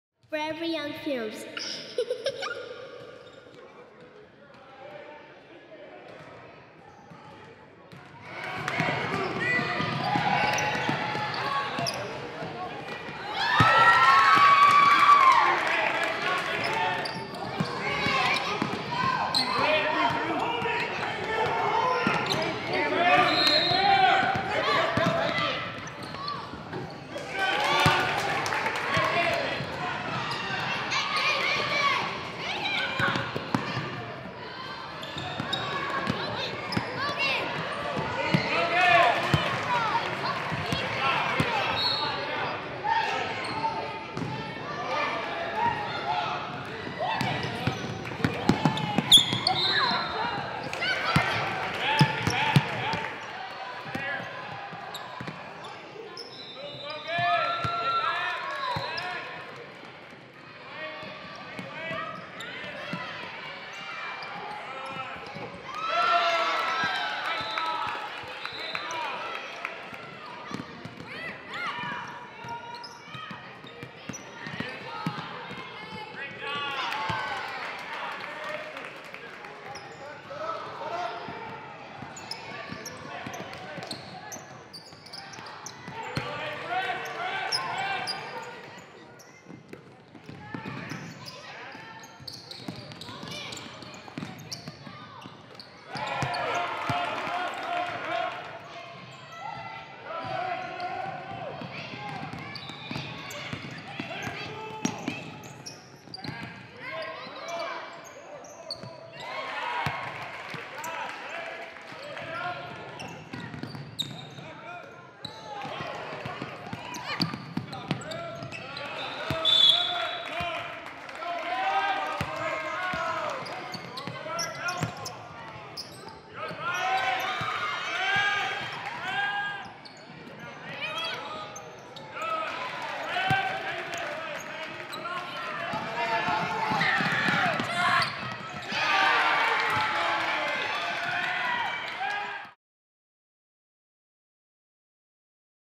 Alodia Tournament | North Houston Hustle 2028
North Houston Hustle continues to look good and improve. A true testament to their coaching staff and their organization as a whole, who stays dedicated to the players and assist them in their development.